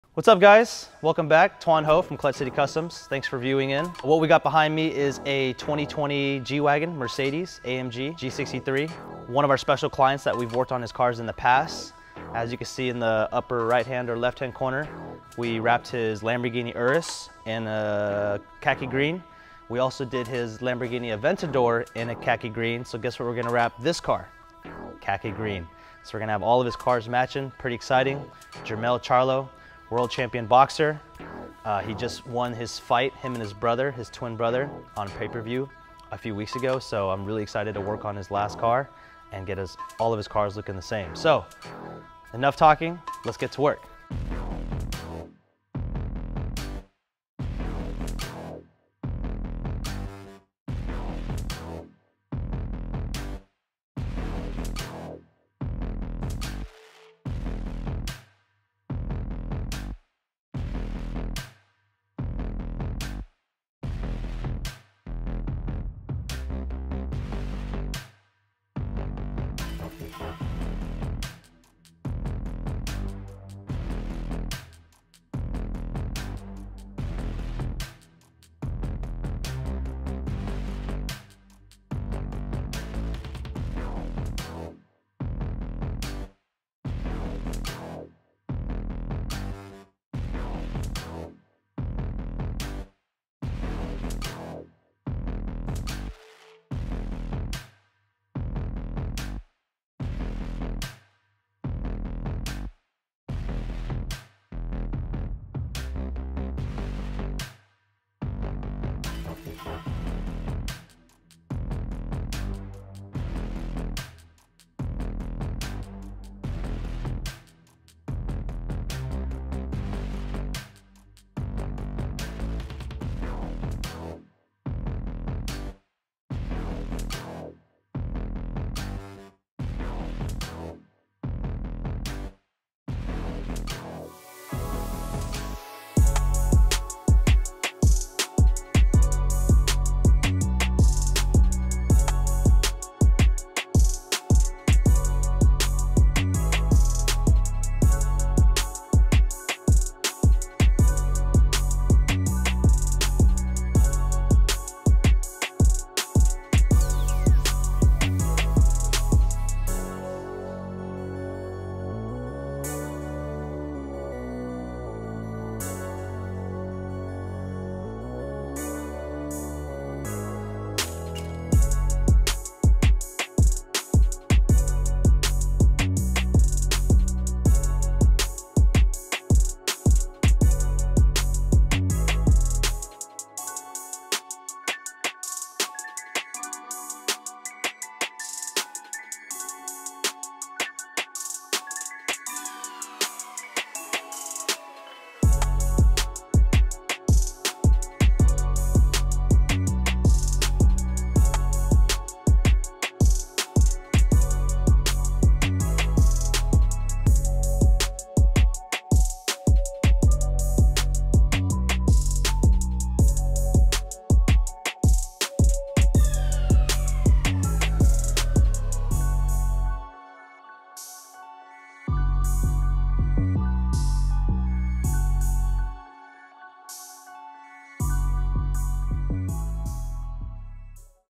We wrapped the new G wagon!
2020 G63 Amg wrapped in Inozetek super gloss Khaki green.

Previously white so we wrapped up the door jambs and hatch jambs as well!